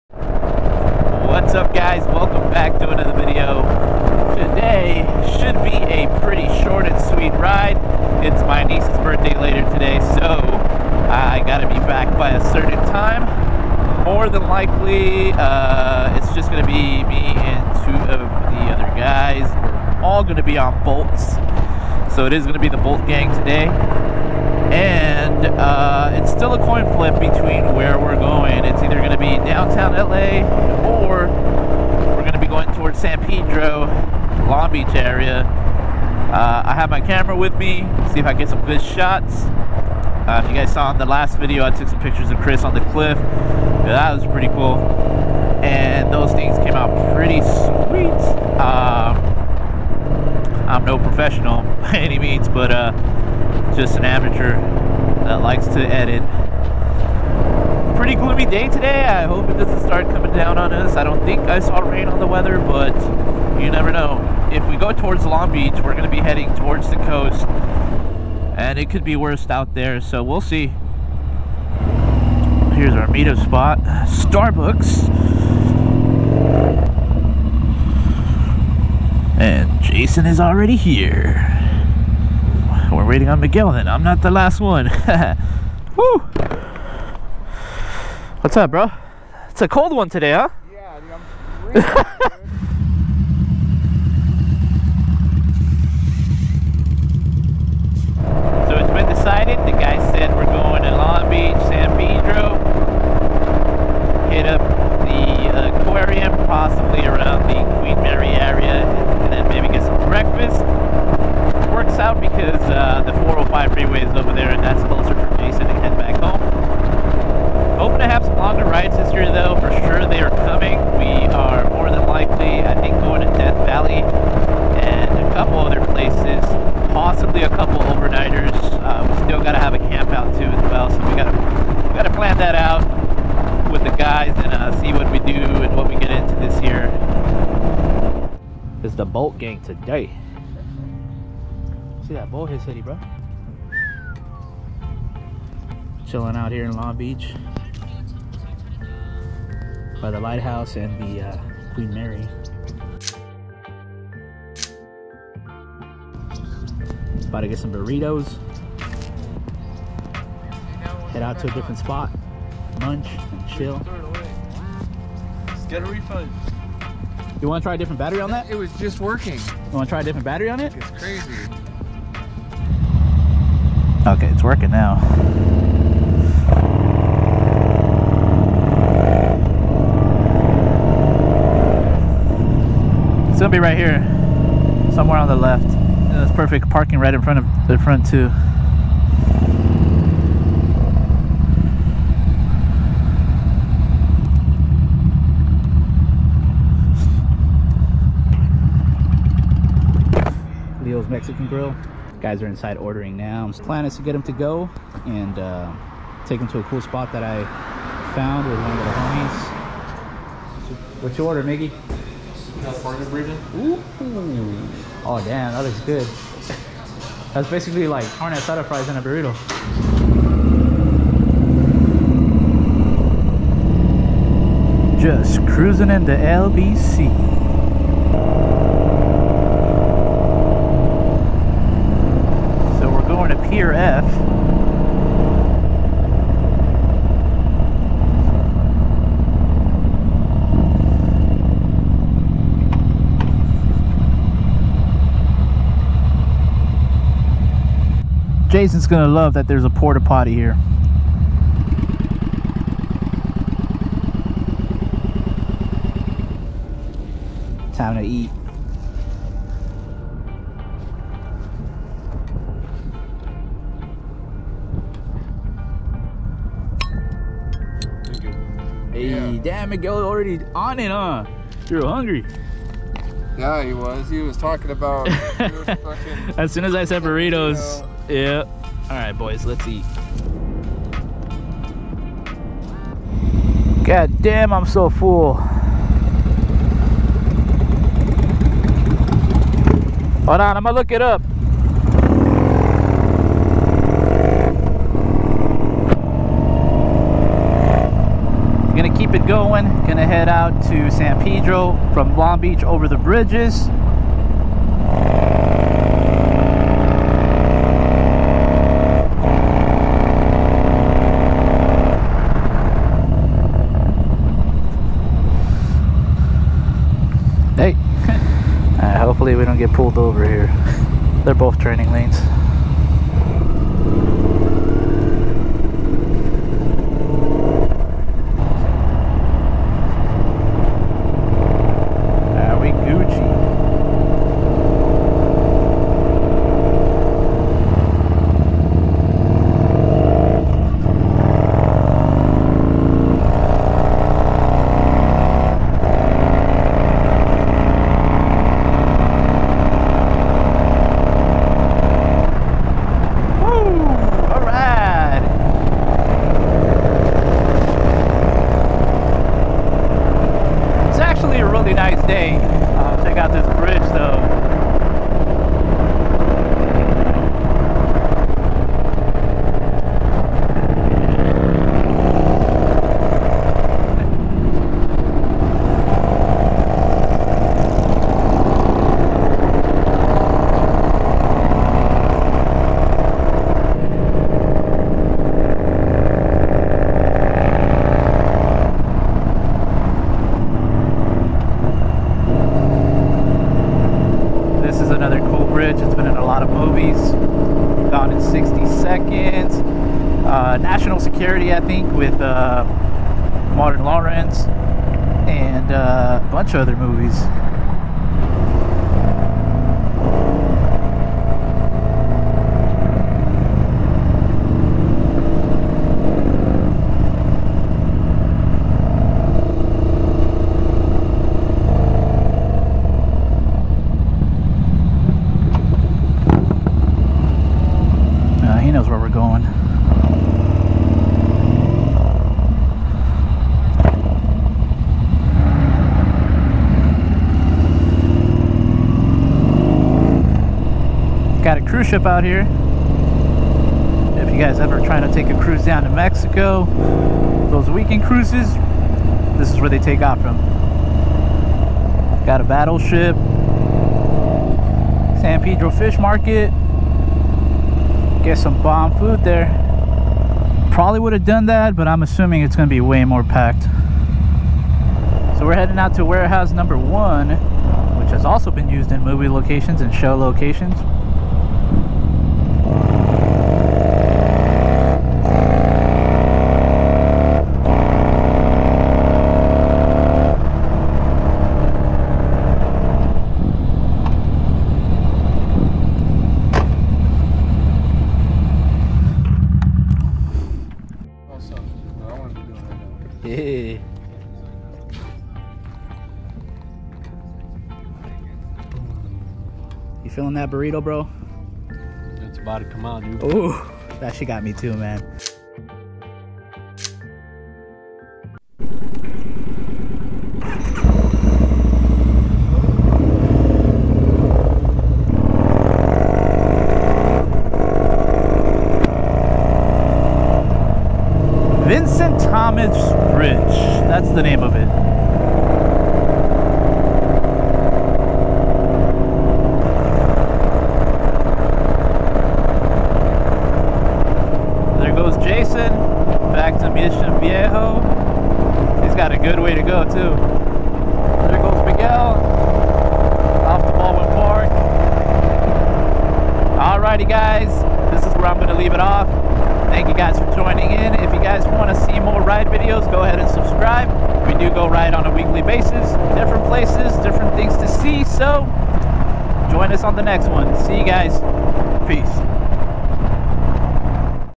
Cruising around on our Yamaha Bolts - Long Beach / San Pedro
Chill ride with the guys, picked up some food and hit up a couple of cool spots.